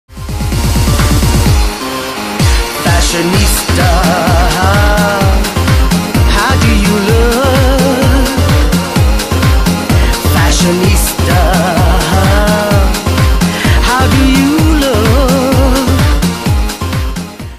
Fashionista Olivia
Olivia wearing her Moms shoes